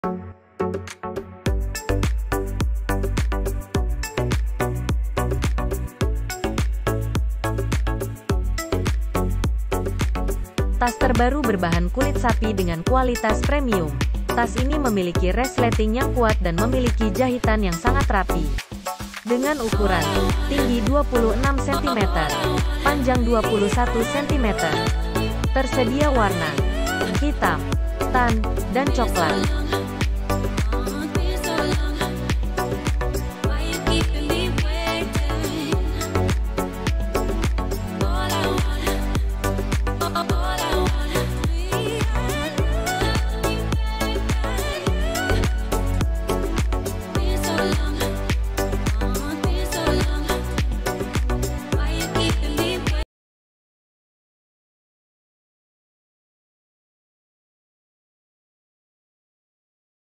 Tas Kulit Asli Indonesia - Tas Kulit Pria
spesifikasi:
- 100% kulit asli premium quality
- memiliki resleting yang kuat
- memiliki jahitan yang sangat rapi
- memiliki kunci tas yang terbuat dari magnet
- tersedia ruang smartphone
- tersedia 2 ruang penyimpanan
- tersedia slot untuk koin dan nota
- memiliki kapasitas ruang yang cukup luas

Bahan 100% Full Kulit Original Satu Lapis.⁣
Dijamin TIDAK MENGELUPAS Dan TAHAN LAMA.⁣
Kualitas Premium⁣
DAYA KEAWETAAN  HINGGA 5TH ++⁣